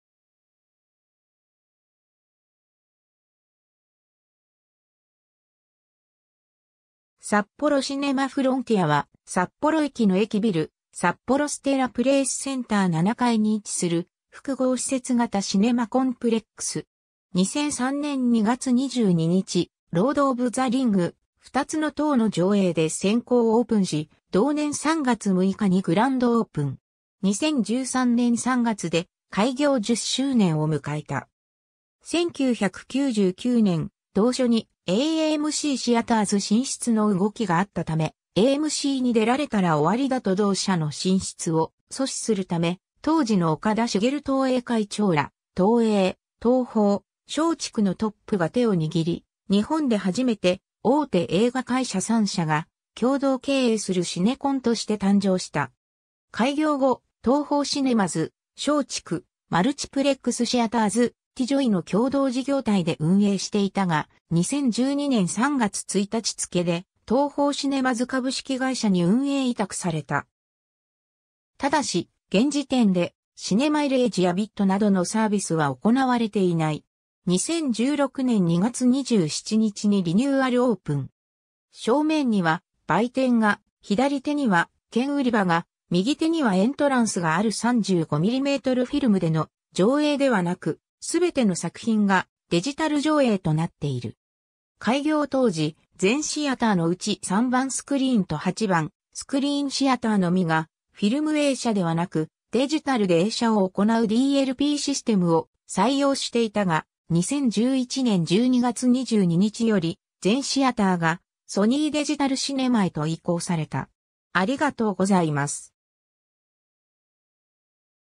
札幌シネマフロンティア
札幌シネマフロンティア（さっぽろシネマフロンティア、Sapporo Cinema Frontier）は、札幌駅の駅ビル札幌ステラプレイスセンター7階に位置する複合施設型シネマコンプレックス。
 2003年2月22日、『ロード・オブ・ザ・リング/二つの塔』の上映で先行オープンし、同年3月6日にグランドオープン。
2013年3月で開業10周年を迎えた。
 1999年、同所にAMCシアターズ進出の動きがあったため、AMCに出られたら終わりだと同社の進出を阻止するため、当時の岡田茂東映会長ら、東映、東宝、松竹のトップが手を握り、日本で初めて、大手映画会社3社が共同経営するシネコンとして誕生した。
開業後、TOHOシネマズ（東宝）、松竹マルチプレックスシアターズ（松竹）、ティ・ジョイ（東映）の共同事業体で運営していたが、2012年3月1日付でTOHOシネマズ株式会社に運営委託された。
ただし現時点でシネマイレージやvitなどのサービスは行われていない。
2016年2月27日にリニューアルオープン。
 正面には売店（コンセッション）が、左手には券売り場（チケットボックス）が、右手にはエントランスがある 35mmフィルムでの上映ではなく、すべての作品がデジタル上映となっている。
 開業当時、全シアターのうち3番スクリーン（259席）と8番スクリーン（455席）シアターのみが、フィルム映写ではなくデジタルで映写を行うDLPシステムを採用していたが、2011年12月22日より全シアターがソニーデジタルシネマへと移行された。